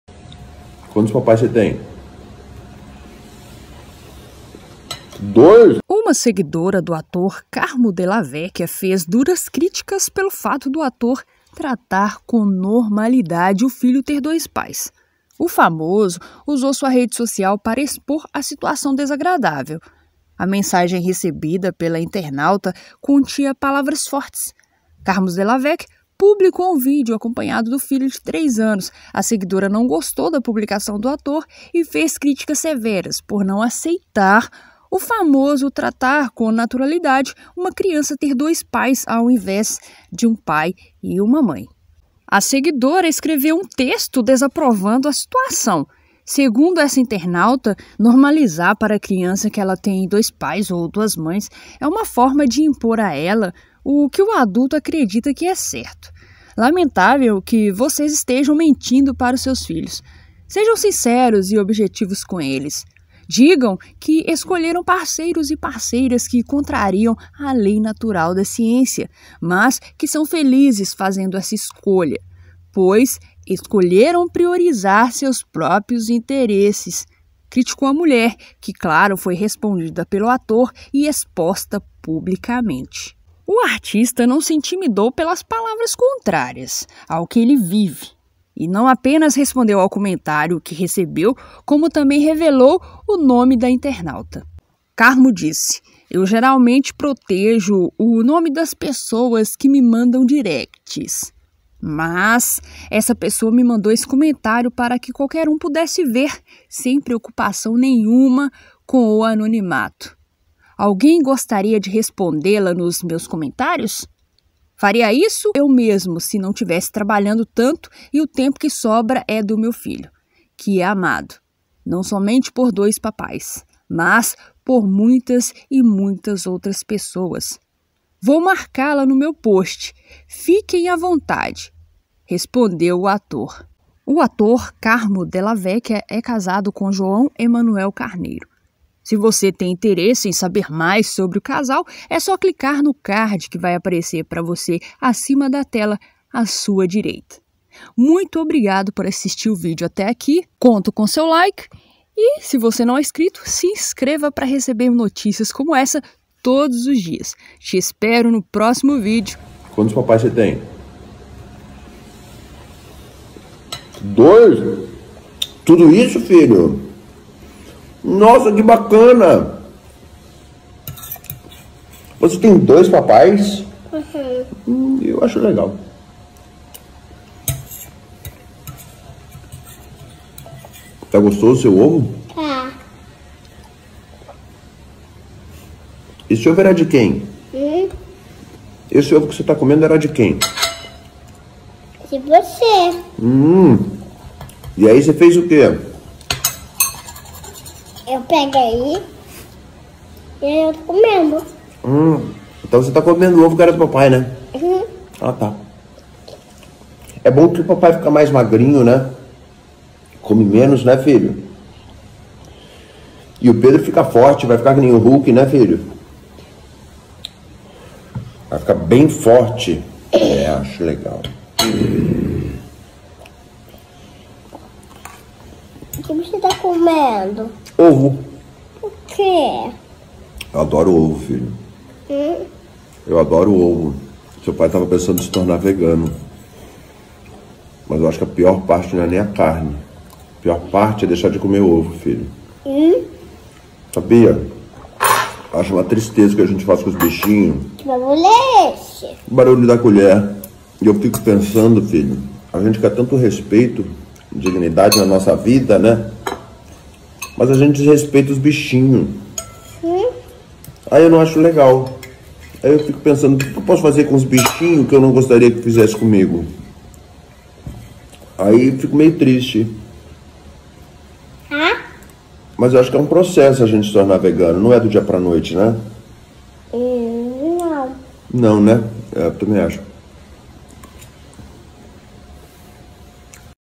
Carmo Dalla Vecchia RESPONDE comentário DESAGRADÁVEL de internauta
Uma seguidora do ator Carmo Dalla Vecchia fez duras críticas pelo fato do ator  tratar com normalidade o filho ter dois pais.
O famoso ,usou sua rede social,para expor a situação desagradável. A mensagem recebida pela internauta continha palavras fortes. Carmo Dalla Vecchia publicou um video acompanhado do filho de 3 anos. A seguidora não gostou da publicação do ator e fez  críticas severas por não aceitar o famosos tratar com naturalidade uma criança ter dois pais aos invés de um pai e uma mãe.
A seguidora escreveu um texto desaprovando a situação,segundo essa internauta:
"Normalizar para a criança que ela tem dois pais ou duas mães é uma forma de impor a ela o que o adulto acredita que é certo. [...] Lamentável que vocês estejam mentindo para os seus filhos, sejam sinceros e objetivos com eles, digam que escolheram parceiros e parceiras que contrariam a lei natural da ciência, mas que são felizes fazendo essa escolha, pois escolheram priorizar seus próprios interesses". Criticou a mulher que claro foi respondida pelo ator e exposta publicamente.
O artista não se intimidou pelas palavras contrárias ao que ele vive e não apenas respondeu ao comentário que recebeu como também revelou o nome da internauta.
Carmo disse:
"Eu geralmente protejo o nome das pessoas que me mandam directs. Mas essa pessoa me mandou esse comentário para que qualquer um pudesse ver sem preocupação nenhuma com anonimato. Alguém gostaria de respondê-la nos meus comentários? Faria isso eu mesmo se não tivesse trabalhando tanto e o tempo que sobra é do meu filho, que é amado não somente por DOIS PAPAIS, mas por muitas e muitas outras pessoas! Vou marcá-la no meu post. Fiquem à vontade!", respondeu ele.
O ator Carmo Dalla Vecchia é casado João Emanuel Carneiro. Se você tem interesse em saber mais sobre o casal é só clicar no card que vai aparecer pra você acima da tela a sua direita.

👩‍💻 Fale com Virou Notícia Oficial 👨‍💻